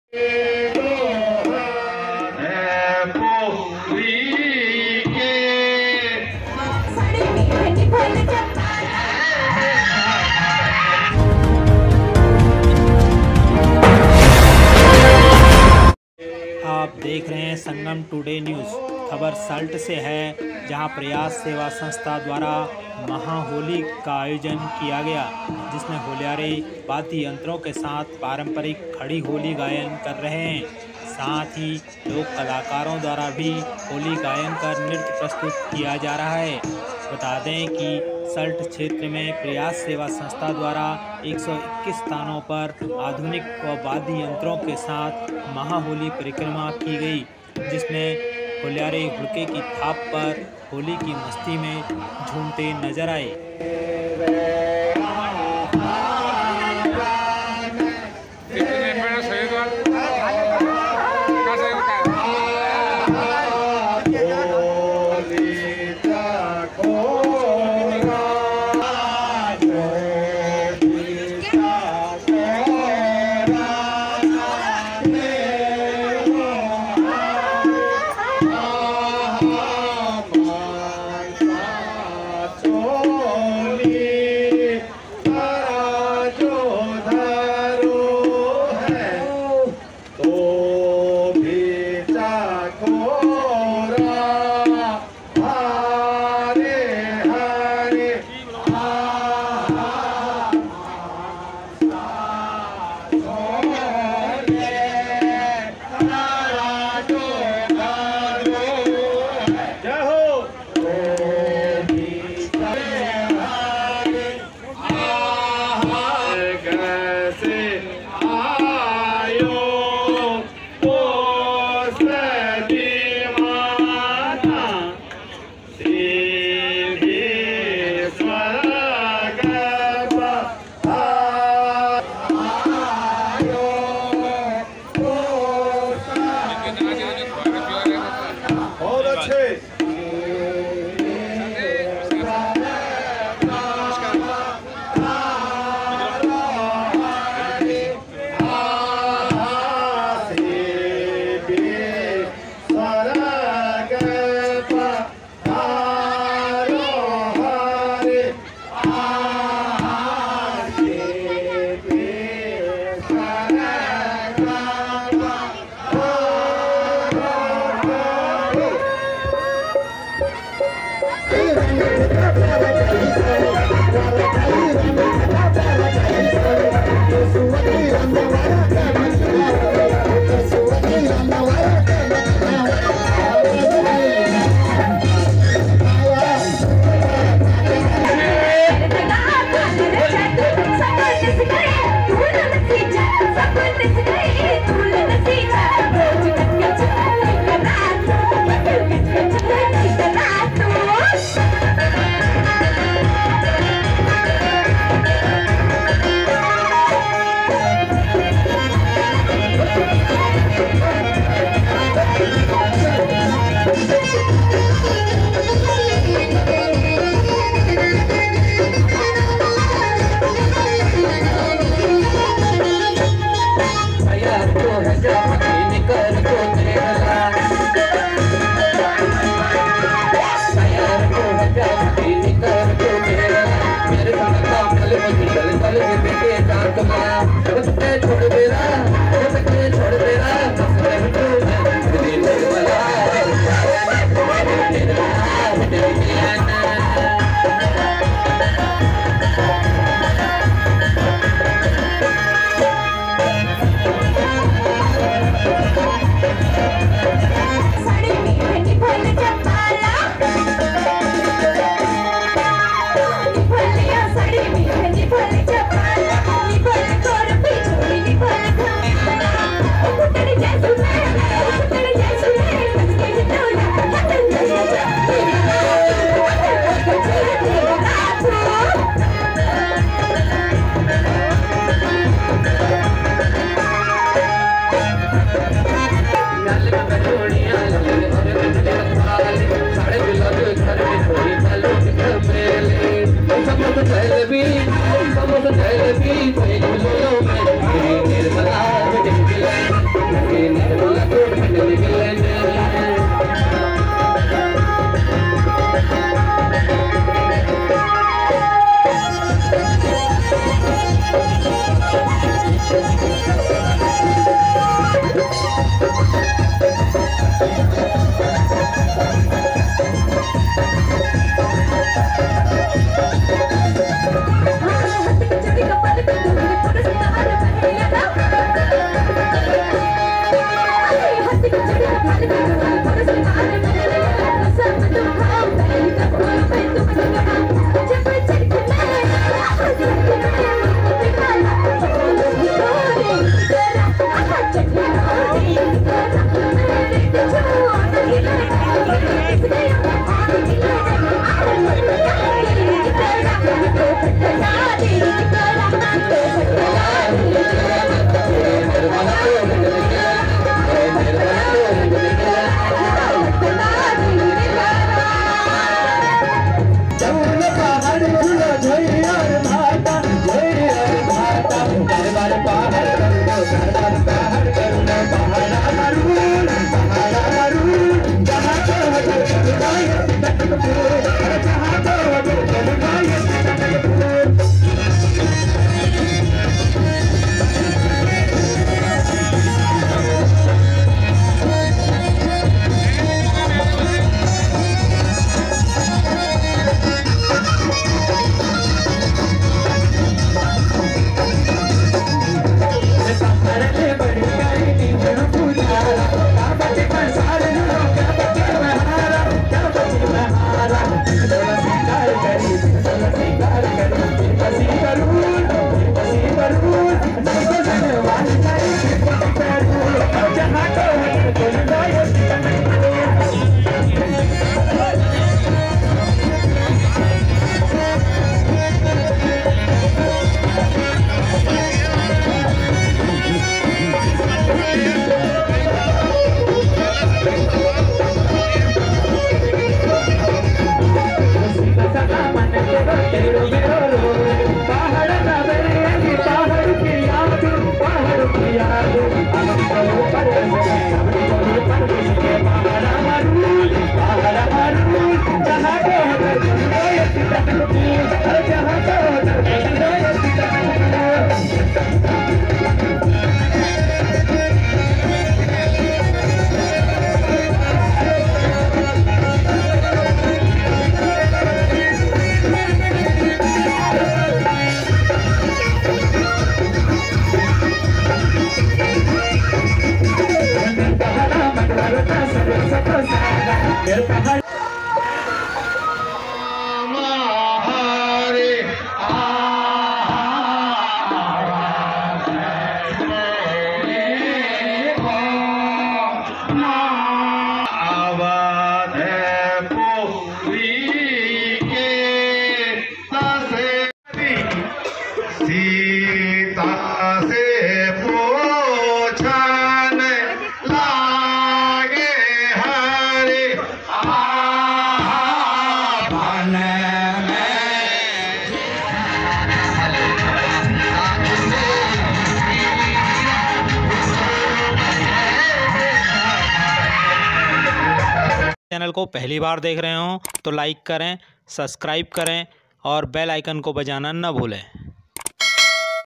सल्ट की महाहोली परिक्रमा,121 स्थानों पर हुआ होली गायन कुछ झलकियां Salt Almora kumaun Holi
सल्ट की महाहोली परिक्रमा,121 स्थानों पर हुआ होली गायन कुछ झलकियां